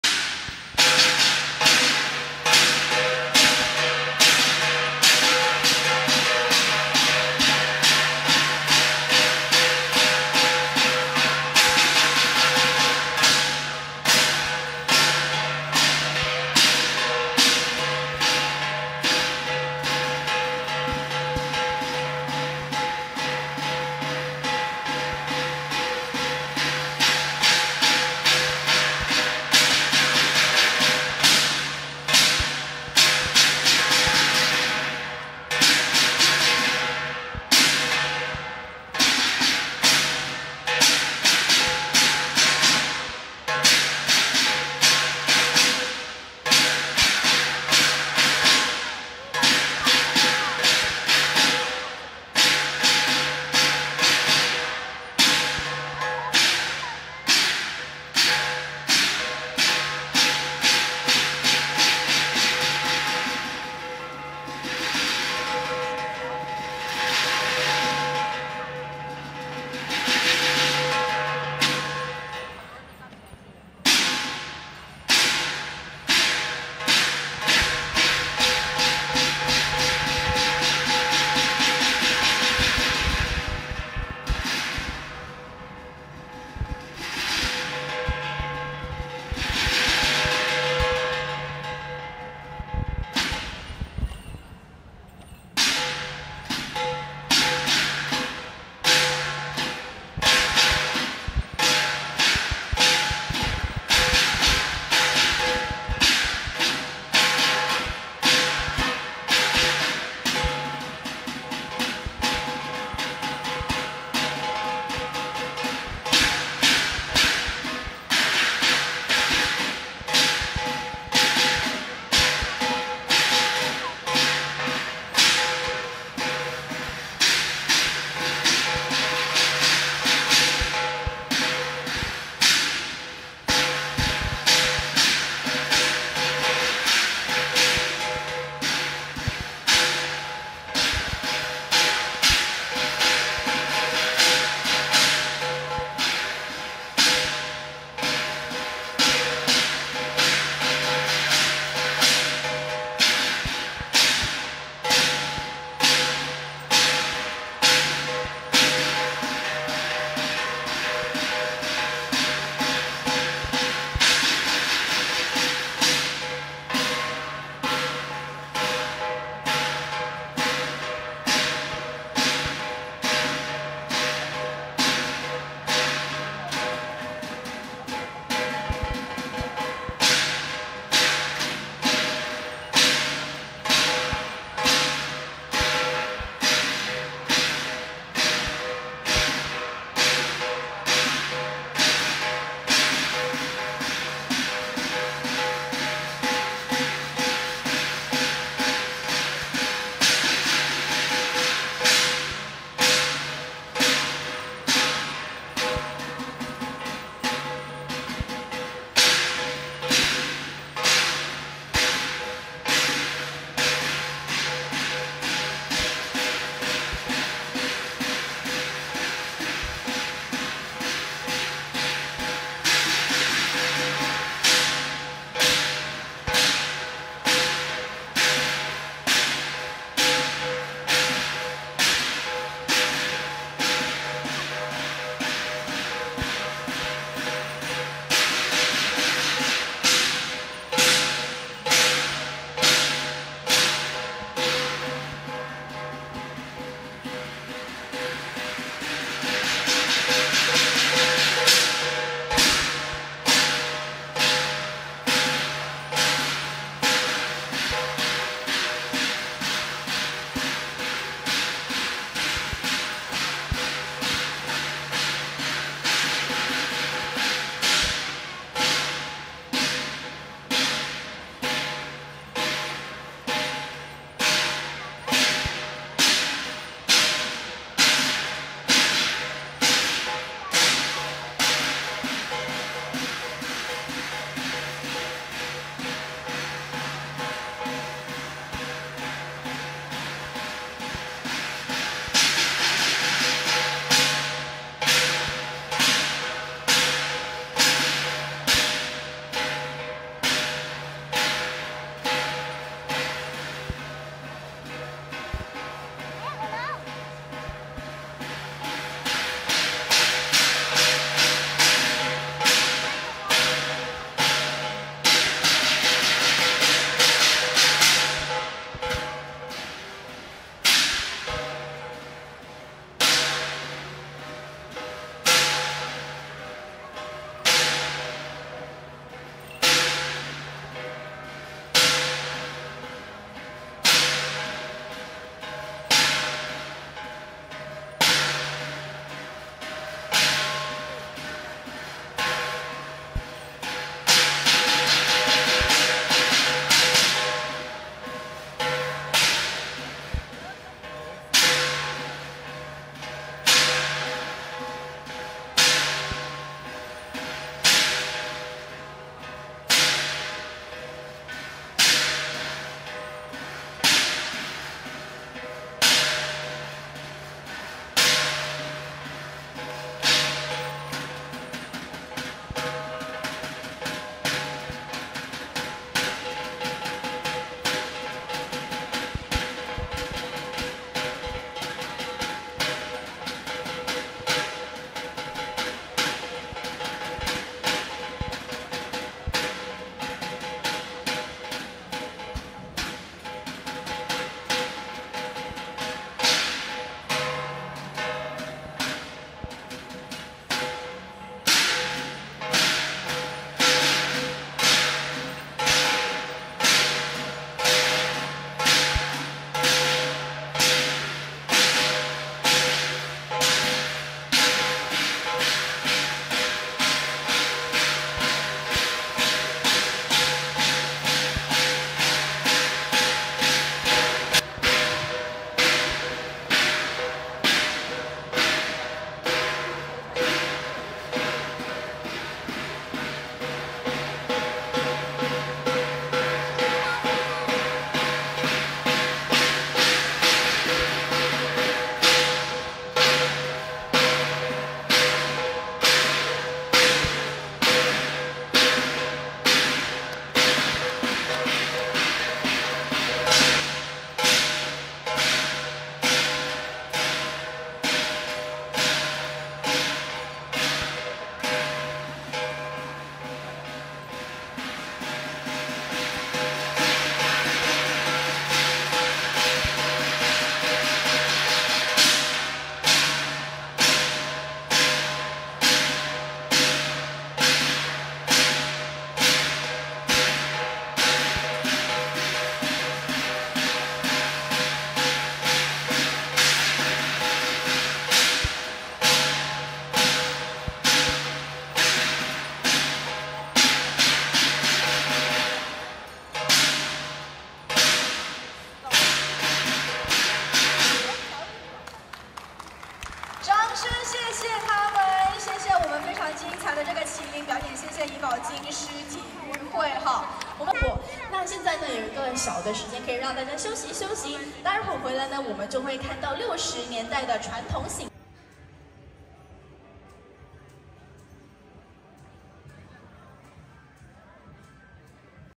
【2023 馬來西亞龙狮节大汇演】World Dragon & Lion Dance Extravaganza (Msia)~ (Unicorn) QiLin dance 怡保金勝麒麟表演 #7
【2023 馬來西亞龙狮节~龙狮瑞獸大汇演】World Dragon & Lion Dance Extravaganza Performances (Msia)~ (Unicorn) QiLin dance  by Ipoh Jing Sheng 馬來西亞怡保金勝麒麟龍獅體育 麒麟表演 Qi Lin Dance Performance@ Lalaport, BBCC, Kuala Lumpur.

 15 Jan 2023

Atraksi barongsai di Malaysia. マレーシアの獅子舞  การเชิดสิงโต របាំនាគ របាំតោ - រាំក្បាច់នាគ múa lân